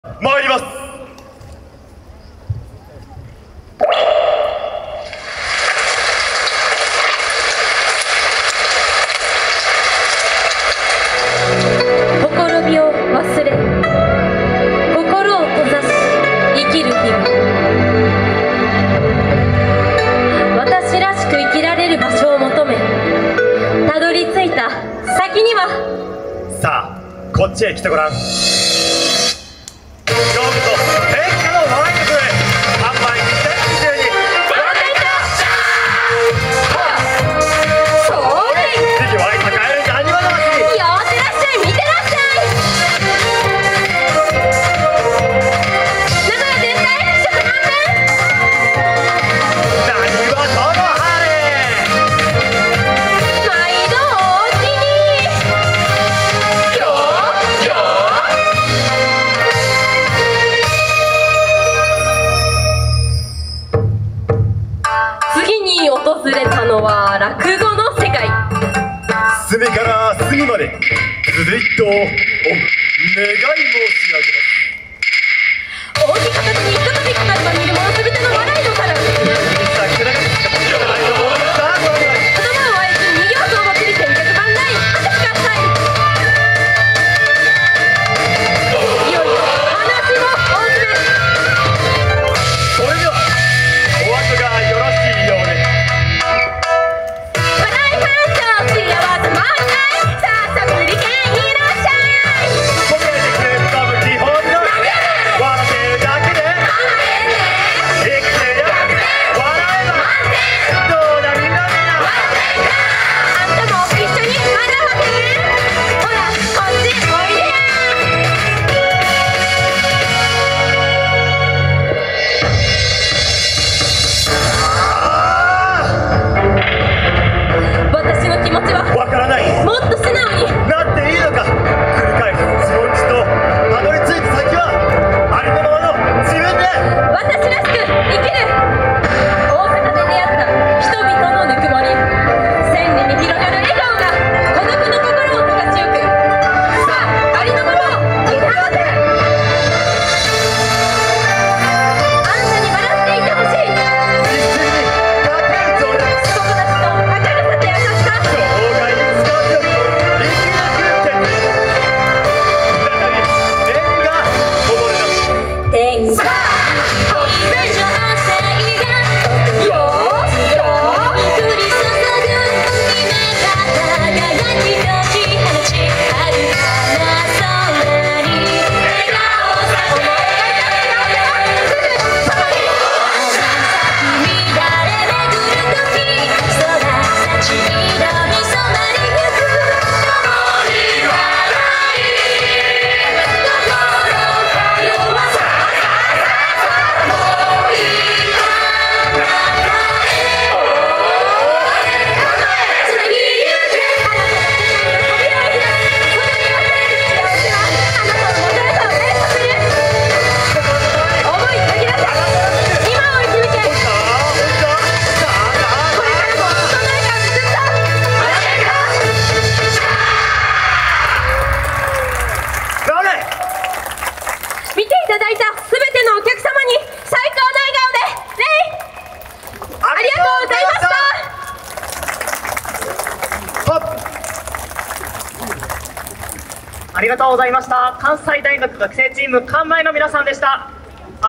関西大学学生チーム"漢舞" 📍にっぽんど真ん中祭り どまつり (ナディアパーク矢場公園会場) 📅2022-08-28T16:24
2022年8月28日(日)、愛知県名古屋市で開催された「にっぽんど真ん中祭り どまつり」(ナディアパーク矢場公園会場)での「関西大学学生チーム""漢舞""」さんの演舞です。